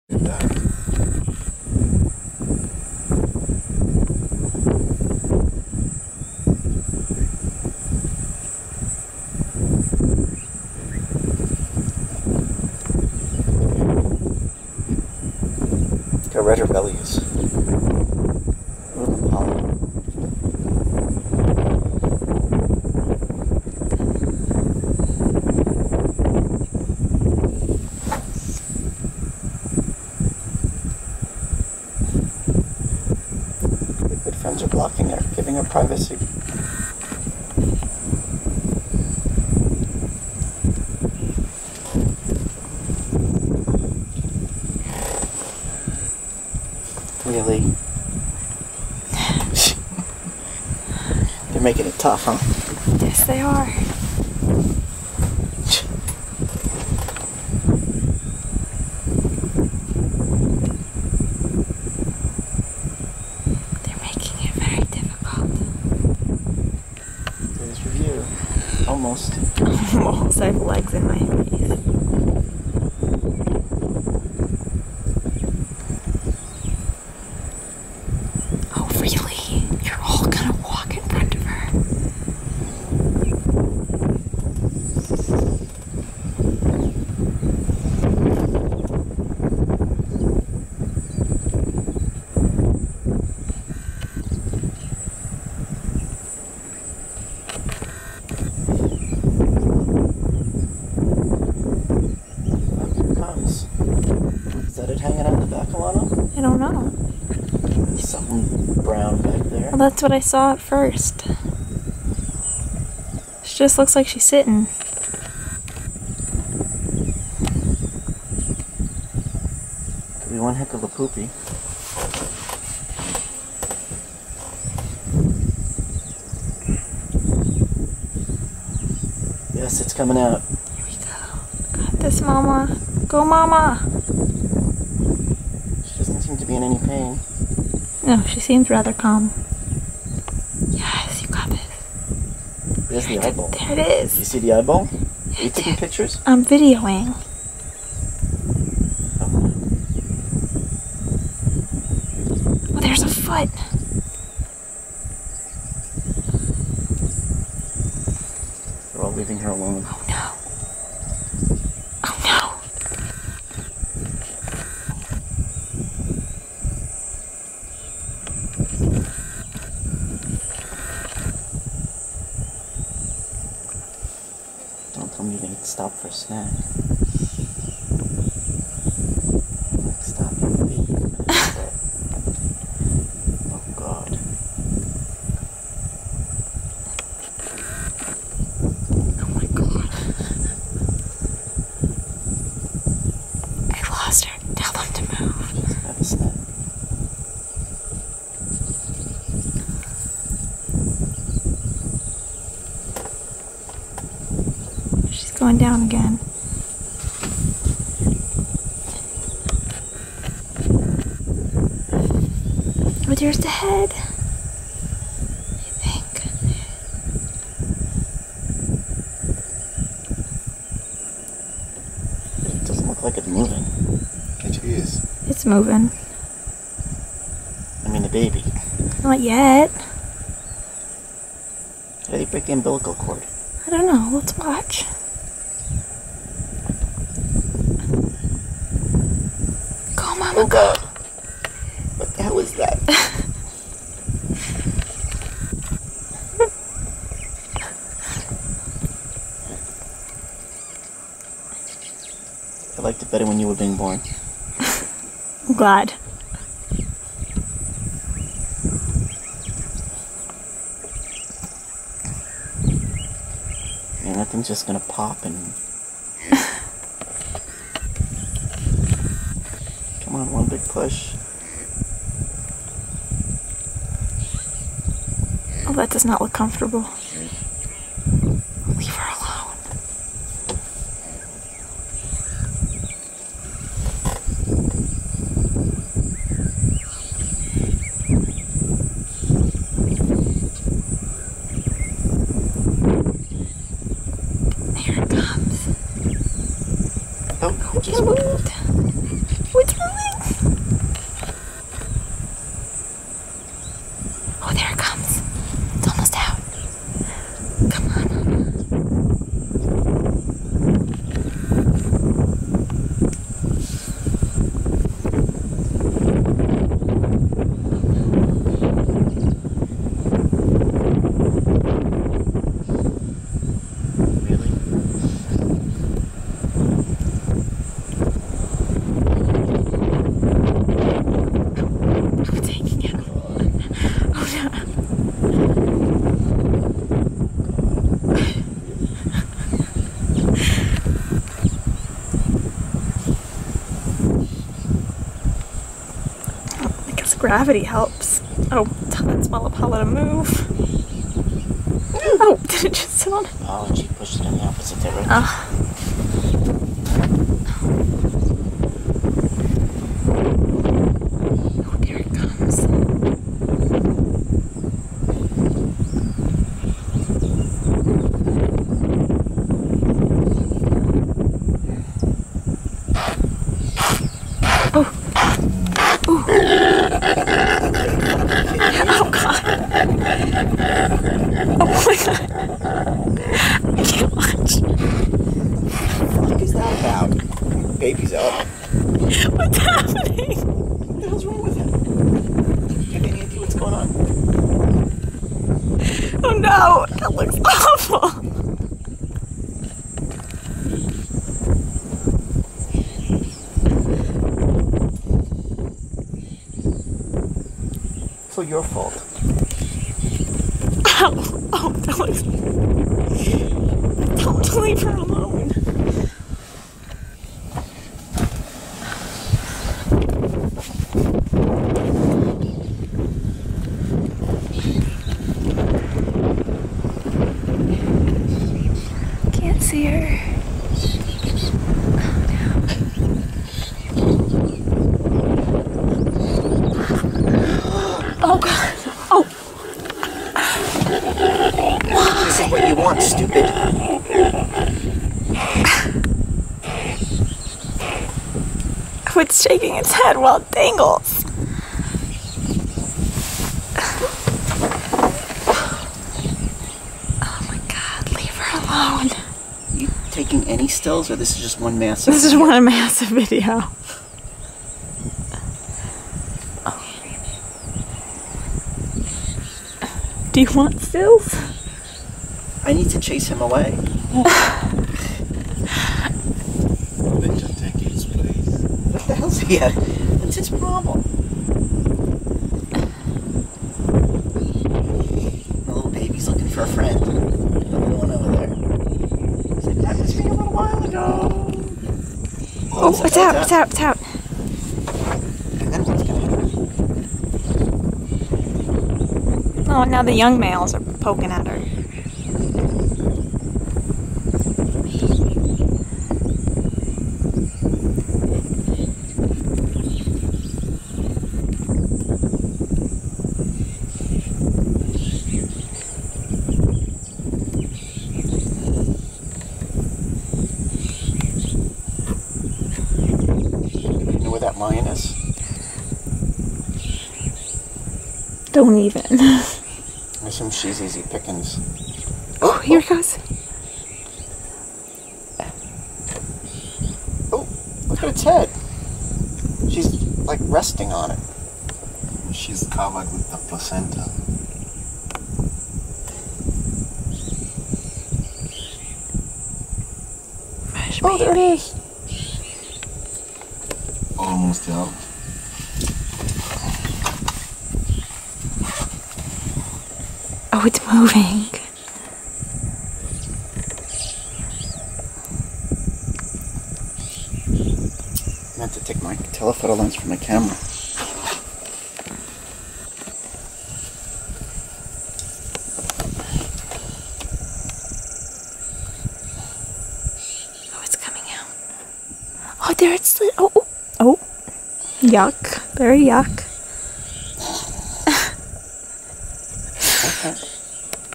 Impala Birth Full Video
Impala giving birth in Serengeti National Park on May 13, 2019.